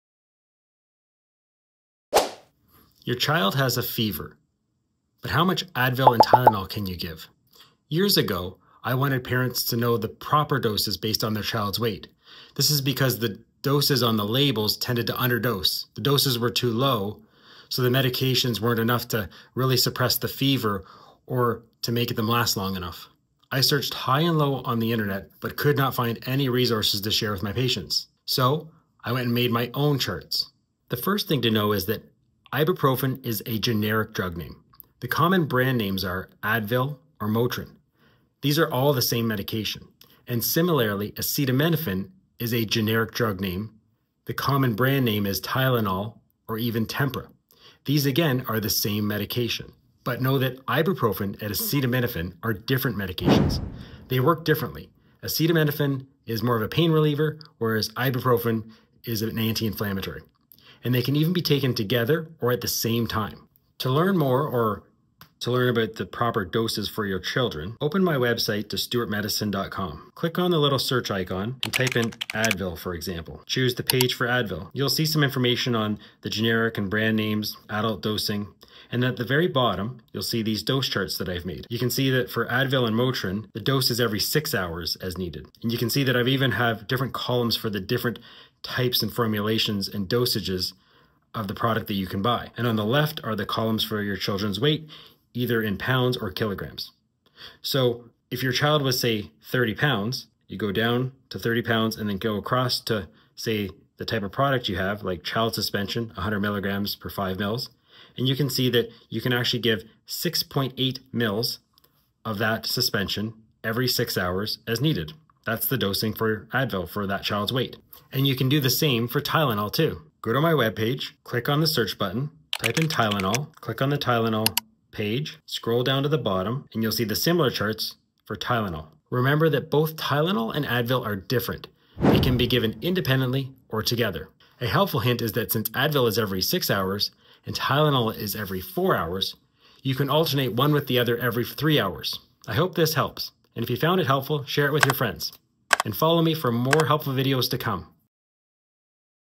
Children Advil and Tylenol Dosing
How much Advil (ibuprofen) and Tylenol (acetaminophen) can you give your child?

The doses on the label are often too LOW.

Years ago, I could not find the proper weight-based dosing anywhere on the internet for my patients. So, I made my own charts.